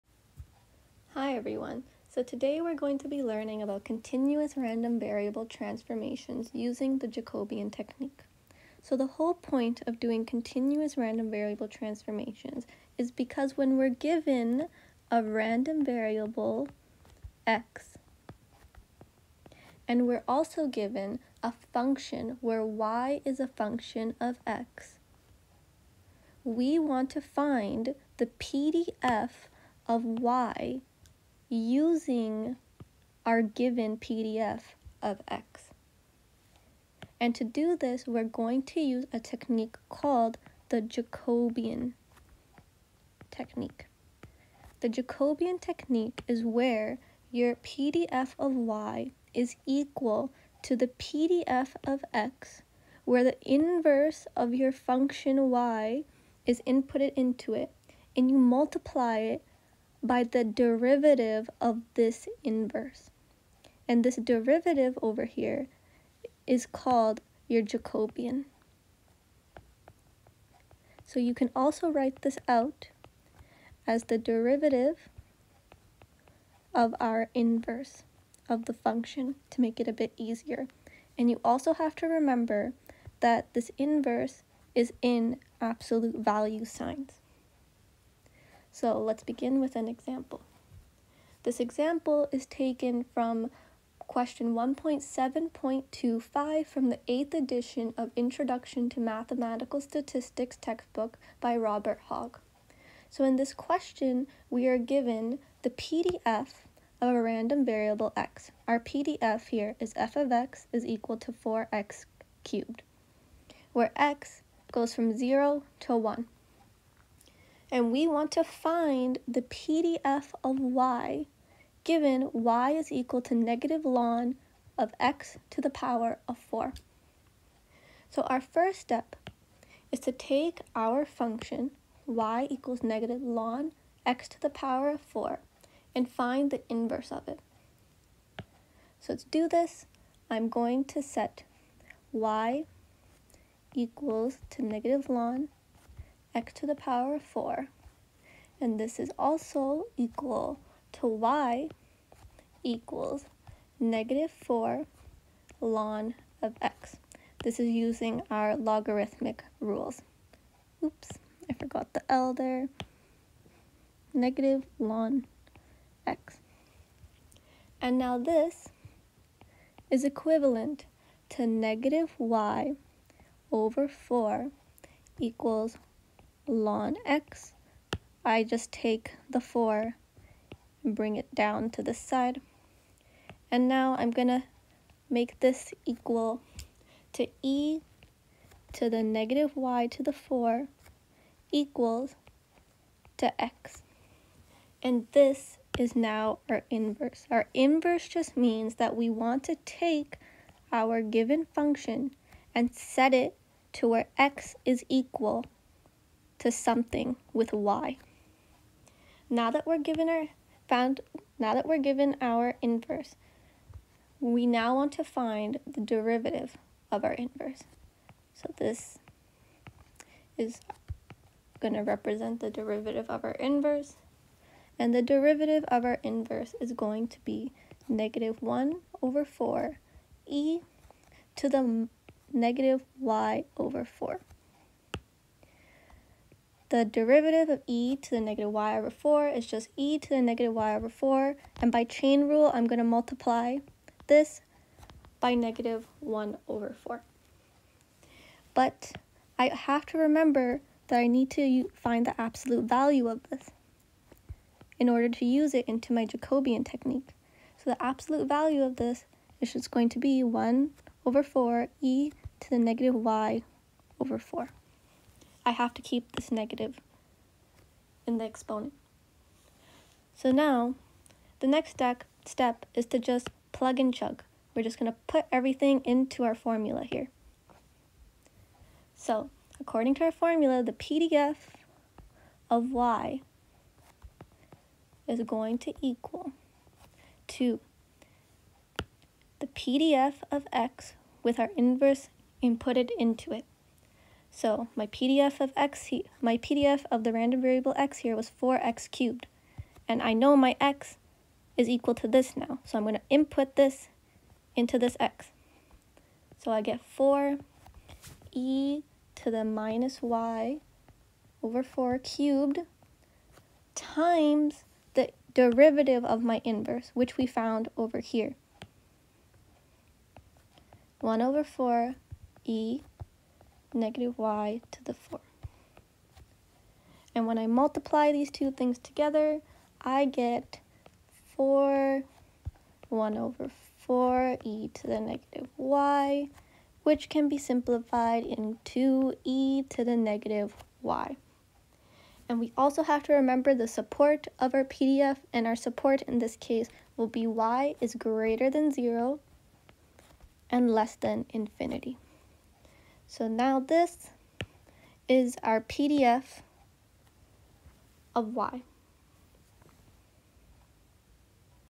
Continuous Random Variables Transformations Jacobian | f(x)=4x^3 & Y=-lnx^4 example
Example from Introduction to mathematical statistics